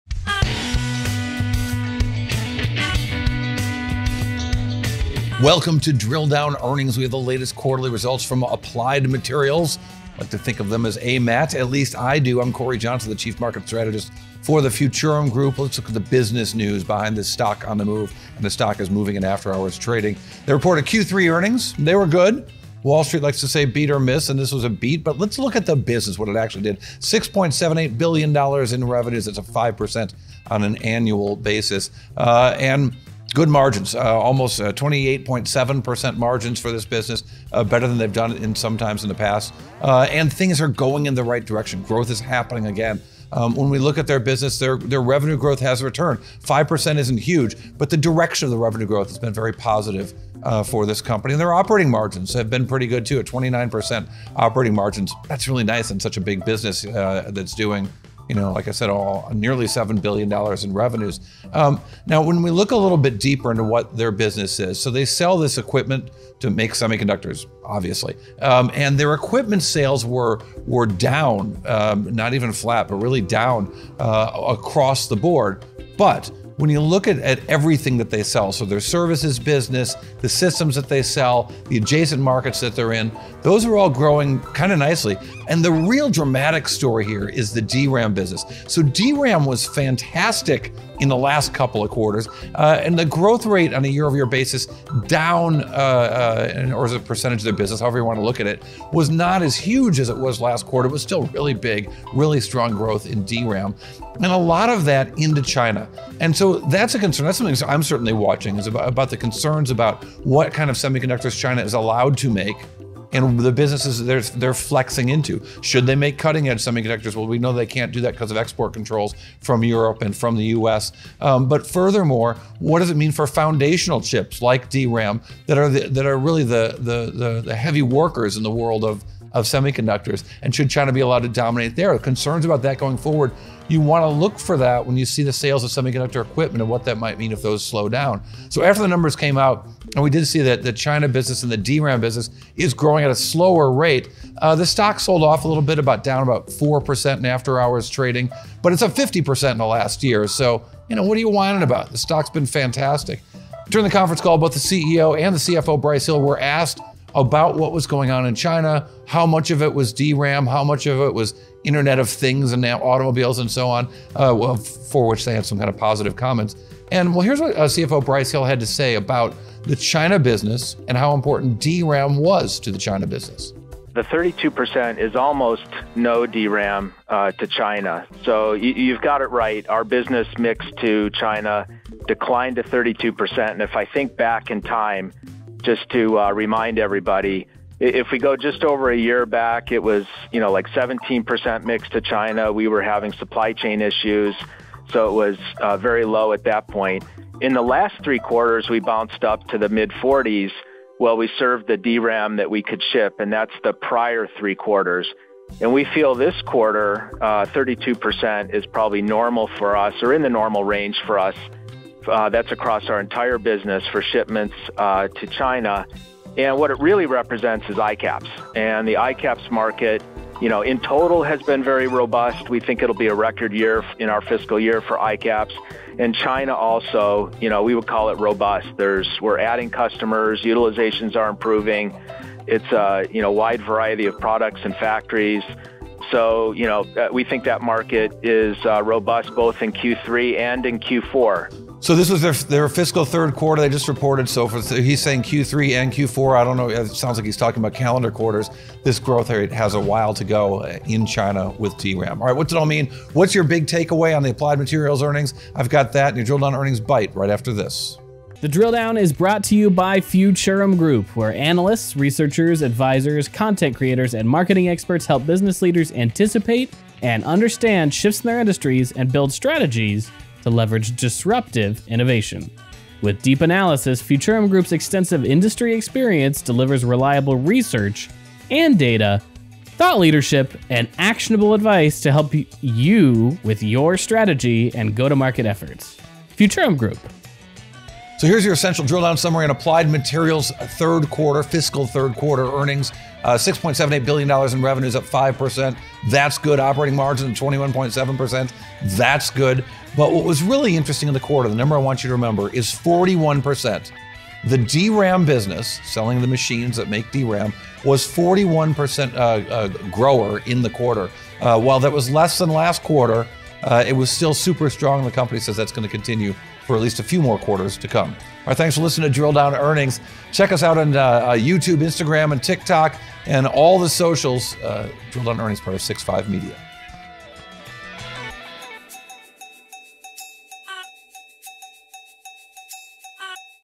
Drill Down Earnings, Ep. 194: Applied Materials Q3 earnings essentials ($AMAT)
Instant analysis of Applied Materials ($AMAT) Q3 earnings, as we hear from CFO Brice Hill.